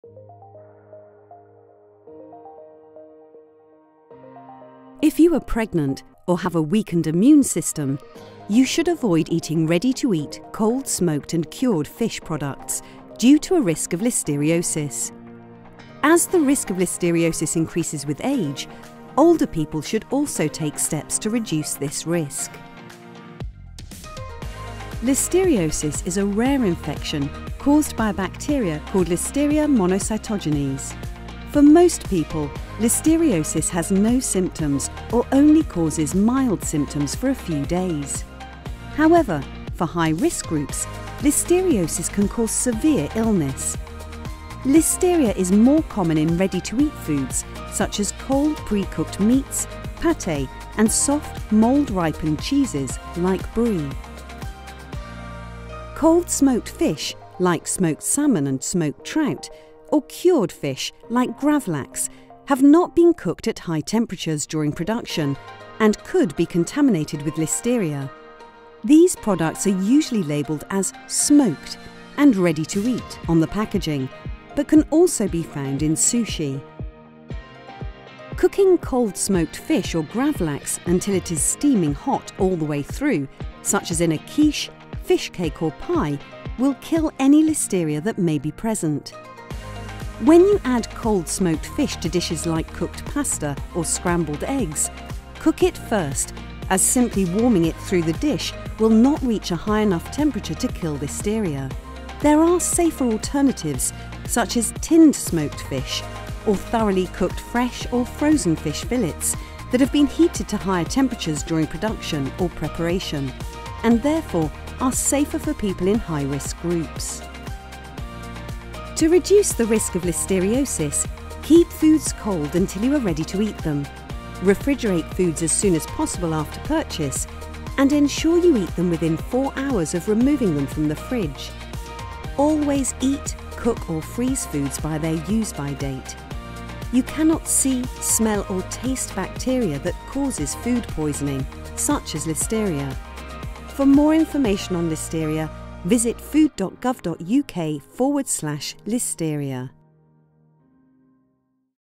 FSA Explains: Listeria in Smoked Fish
Smoked fish advice for higher-risk groups

Pregnant women or people who have a weakened immune system are more likely to suffer severe symptoms from listeria infection and are advised to avoid eating ready-to-eat cold-smoked or cured fish products, such as smoked salmon or gravlax.

As the risk of serious illness from listeriosis increases with age, older people should also be aware of the risk of eating cold-smoked and cured fish and consider taking steps to reduce listeria infection. These steps include eating foods before the use-by dates, ensuring the product is kept refrigerated (below 5°C) or considering using the safer alternatives below.

Ready-to-eat cold smoked fish is normally labelled as ‘smoked’ fish on its packaging. It has been cured (for example in salt) and then smoked at a low temperature, but it is not cooked through. Ready-to-eat cold smoked fish typically comes in thin slices, as shown in the image on this page. Examples include smoked trout slices and smoked salmon slices.

For advice on what you can do to reduce the risk of becoming ill due to listeria